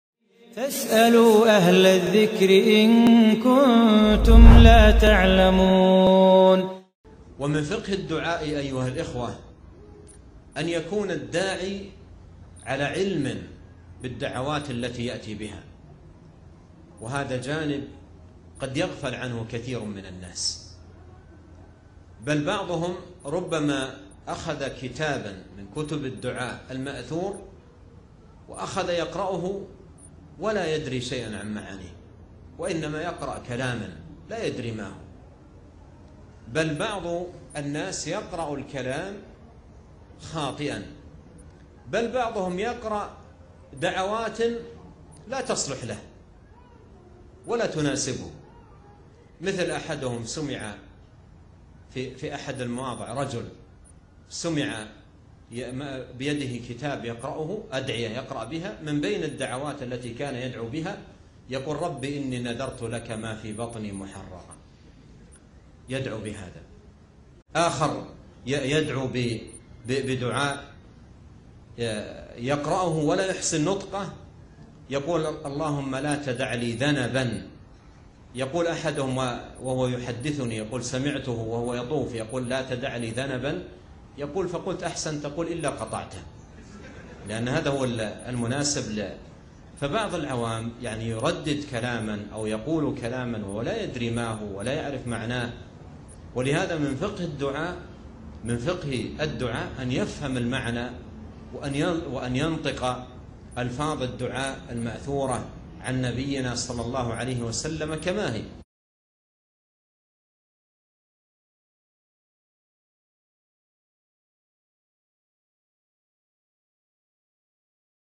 Hati-hati Berdoa Tanpa Paham Maknanya - Syaikh Abdurrozaq Al-Badr
Ibnu Taimiyah berkata :

وَالدُّعَاءُ يَجُوزُ بِالْعَرَبِيَّةِ وَبِغَيْرِ الْعَرَبِيَّةِ وَاَللَّهُ سُبْحَانَهُ يَعْلَمُ قَصْدَ الدَّاعِي وَمُرَادَهُ وَإِنْ لَمْ يُقَوِّمْ لِسَانَهُ فَإِنَّهُ يَعْلَمُ ضَجِيجَ الْأَصْوَاتِ بِاخْتِلَافِ اللُّغَاتِ عَلَى تَنَوُّعِ الْحَاجَاتِ .

“Berdo’a boleh dengan bahasa Arab dan bahasa non Arab. Allah subhanahu wa ta’ala tentu saja mengetahui setiap maksud hamba walaupun lisannya pun tidak bisa menyuarakan. Allah Maha Mengetahui setiap do’a dalam berbagai bahasa pun itu dan Dia pun Maha Mengetahui setiap kebutuhan yang dipanjatkan” (Majmu’ Al Fatawa, 22/489)

INFAQ DONASI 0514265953 BNI Syariah An. Bayu Yudho A